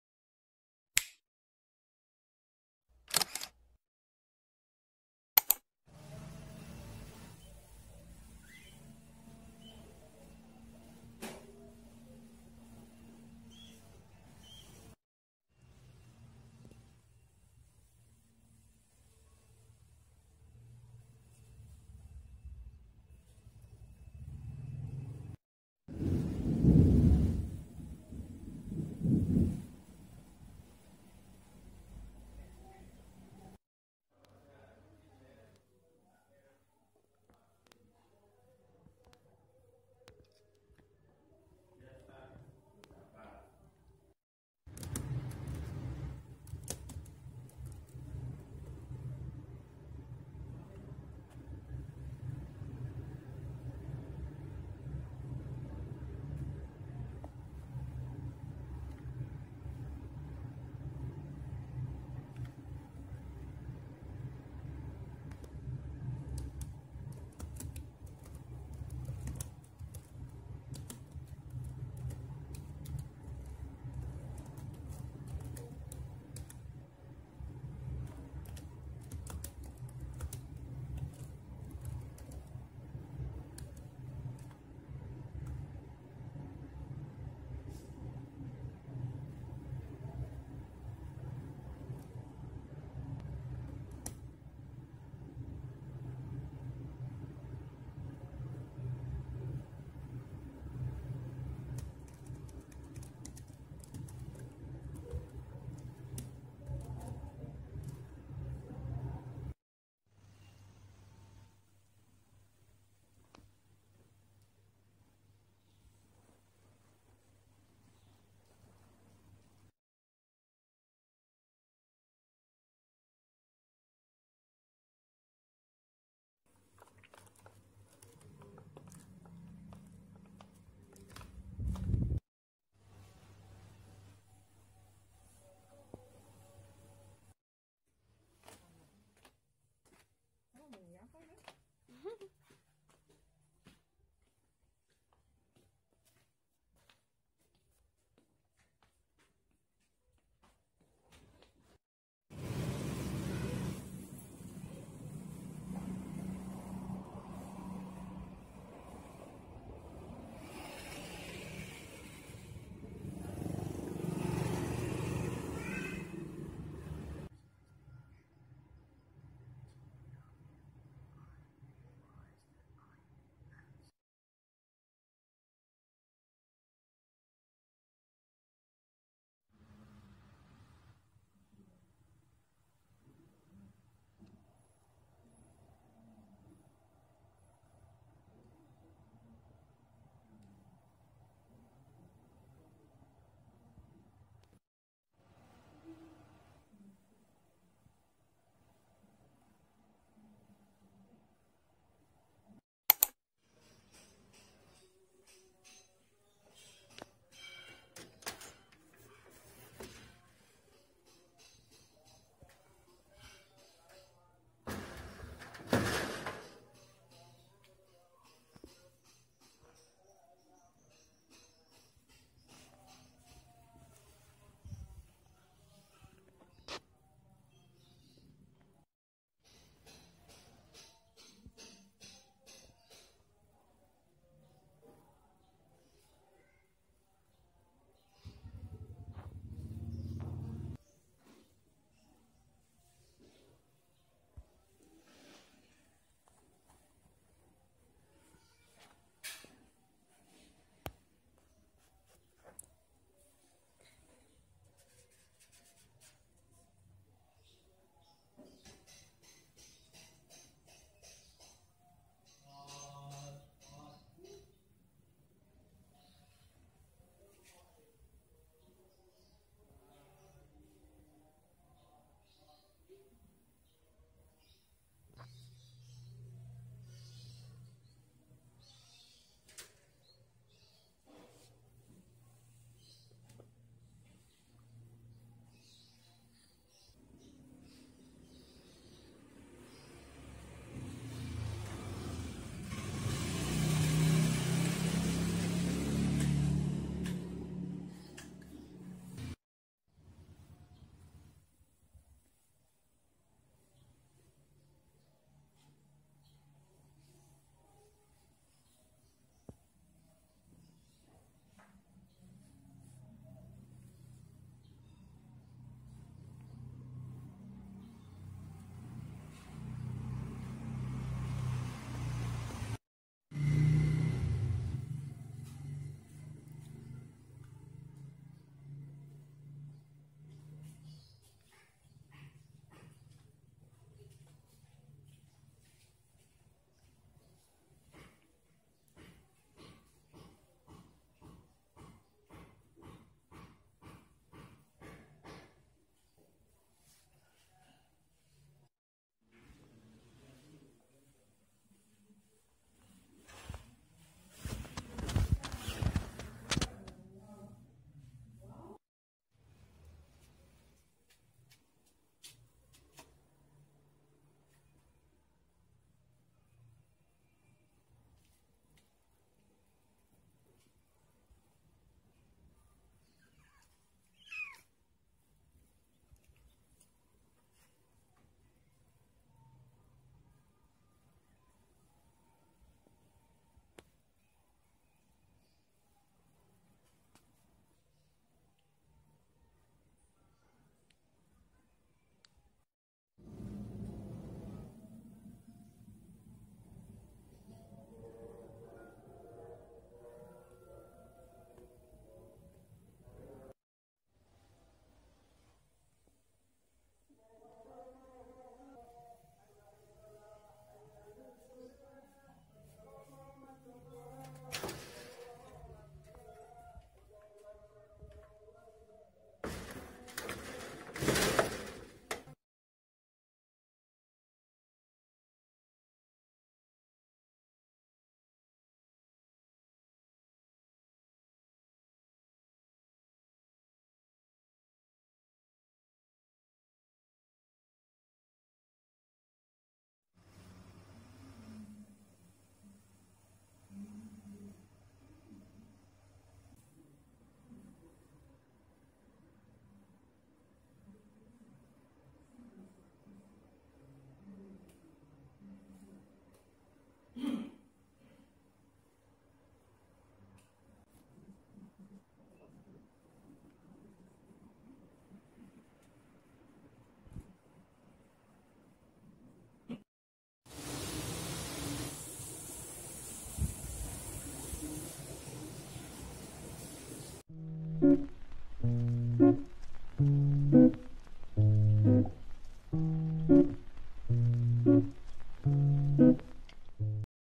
[ENG] quarantine day as a homebody vlog, online class, eating, and nap | indonesia
hello my friends, this is safira!

i slowly forgot about how many days goes by haha.. this is my first "official" video on this channel. i hope you all enjoying it and if you want give some thoughts too in the comment section below :)

have a great day, stay safe and stay healthy!